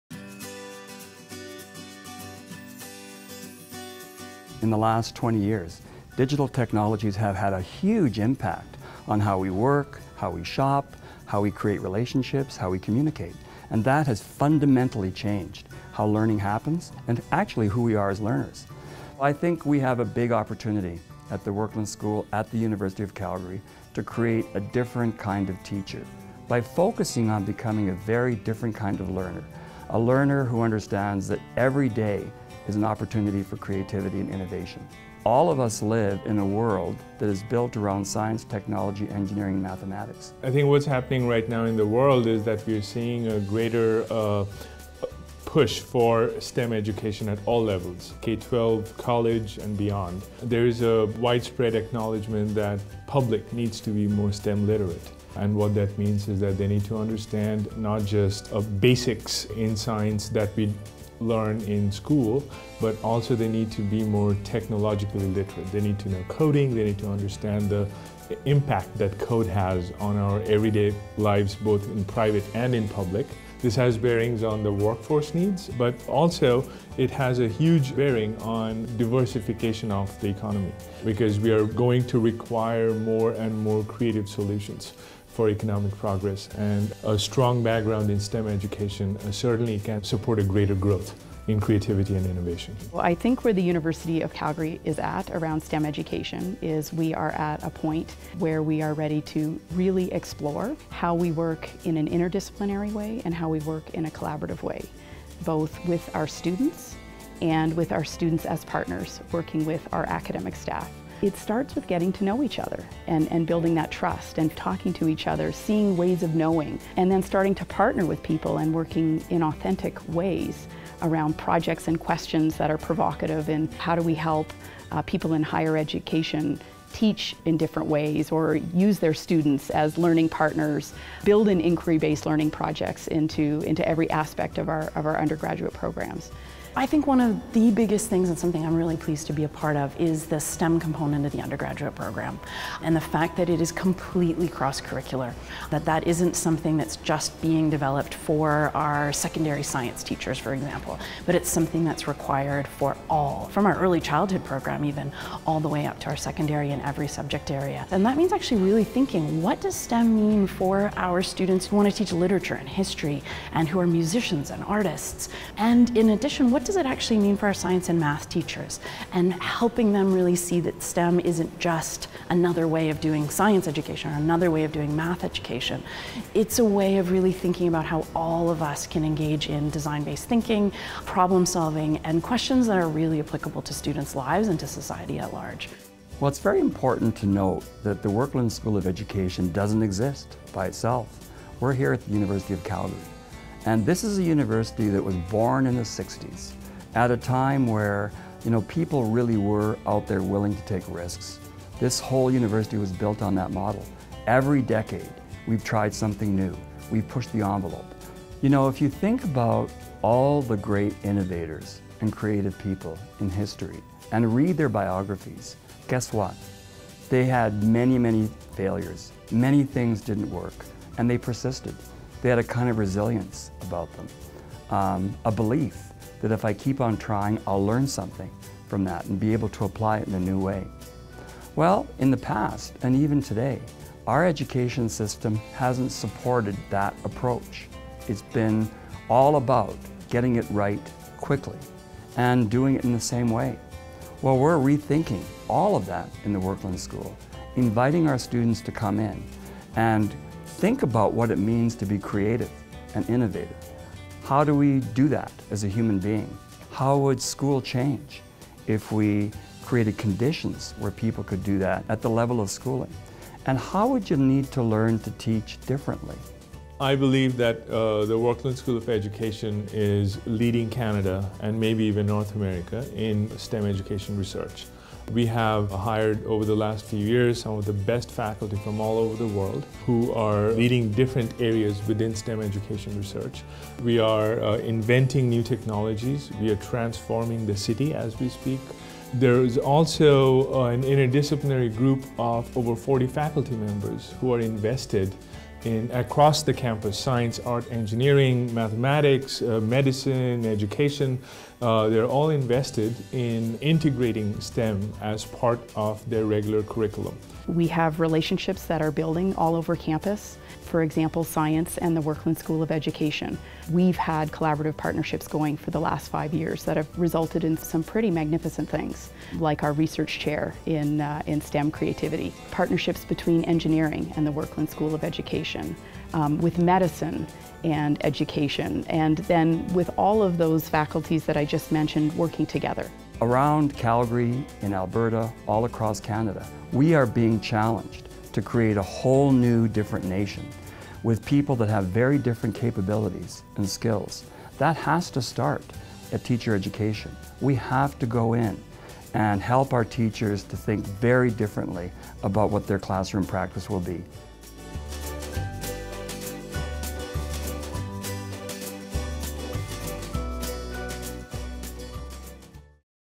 The Future of Learning in STEM Education
University of Calgary academics discuss the importance of STEM education.